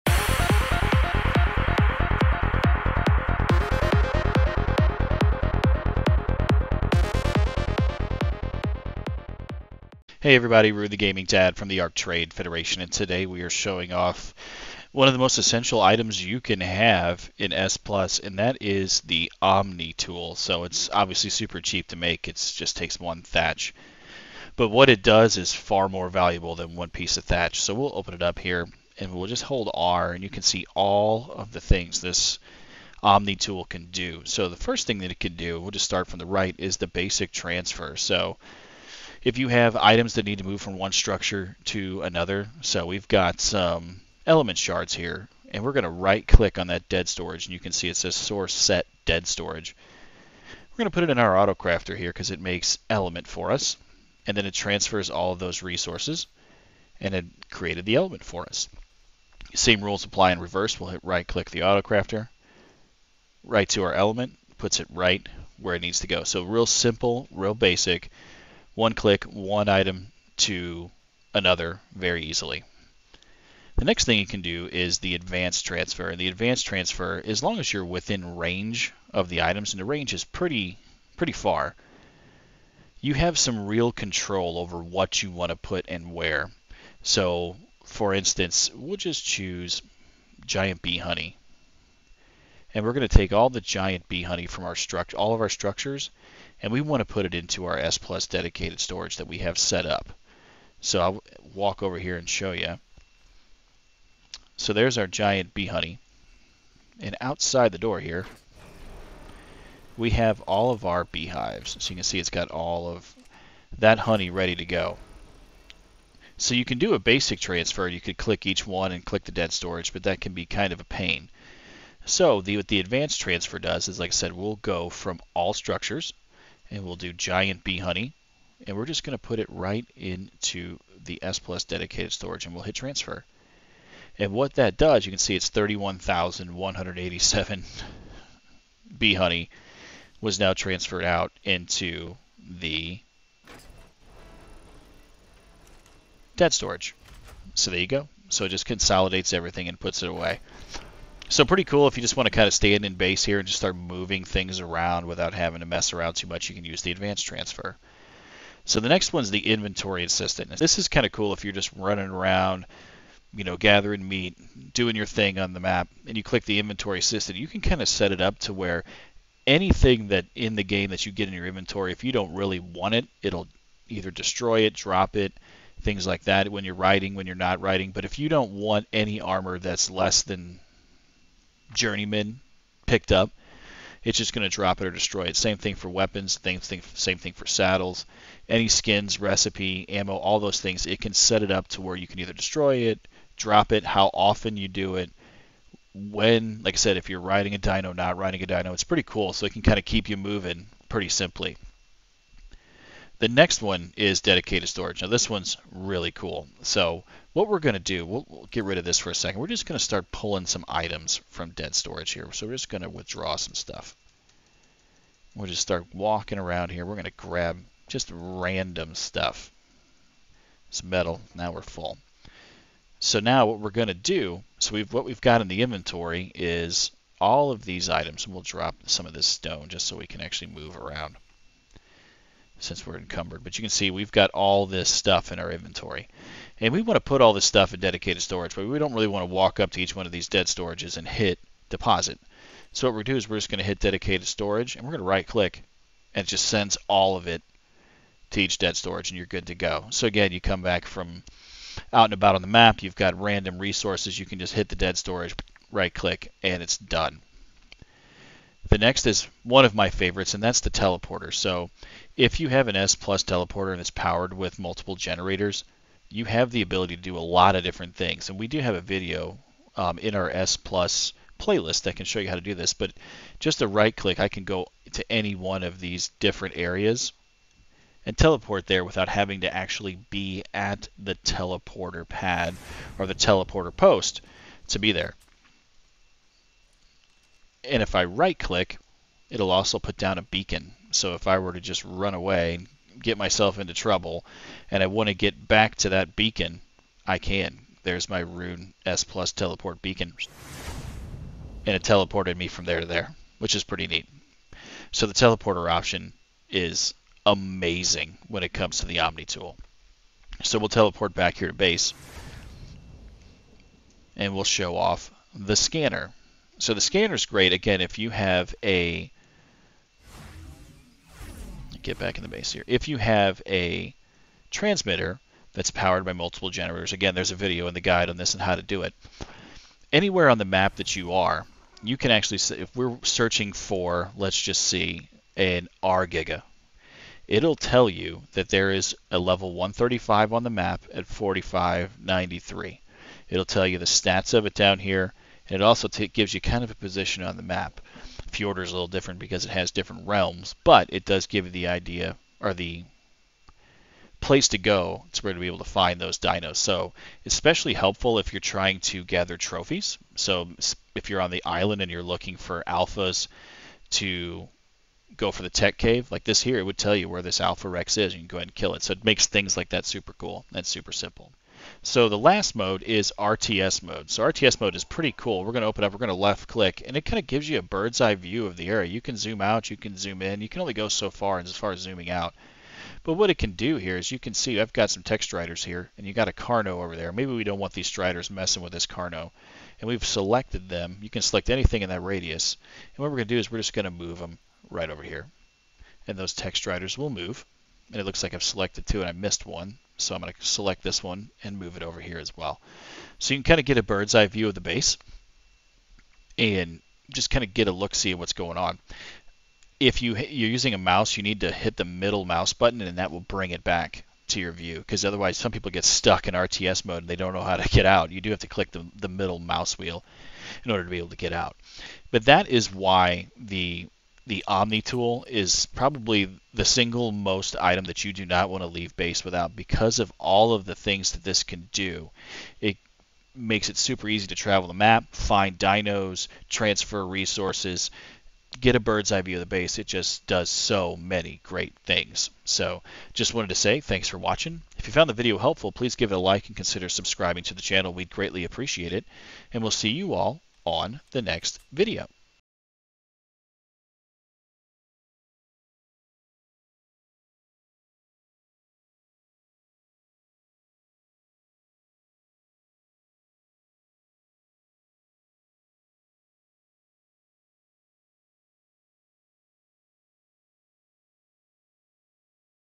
The S+ Omni Tool....Don't leave base without it!!!
A brief walkthrough on the wonders that is the S+ Omni Tool!